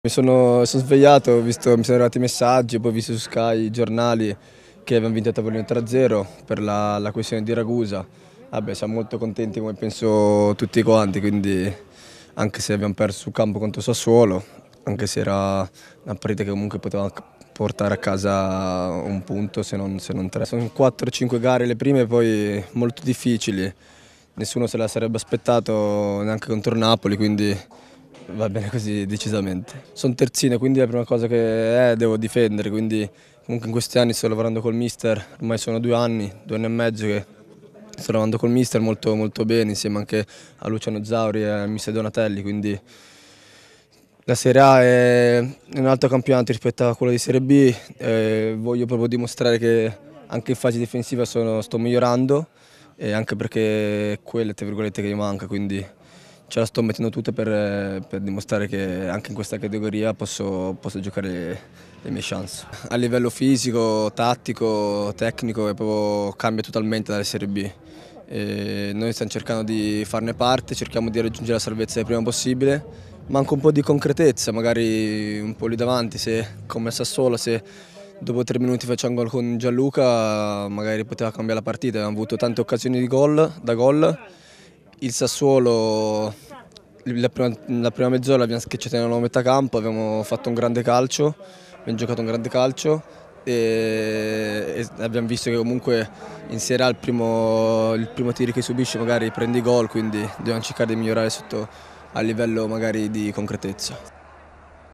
Pescara, Zampano: "Che sorpresa la decisione del giudice"
Francesco Zampano racconta la sorpresa con cui ha accolto stamani la notizia della vittoria a tavolino assegnata dal giudice sportivo al Pescara per la gara di due giorni fa col Sassuolo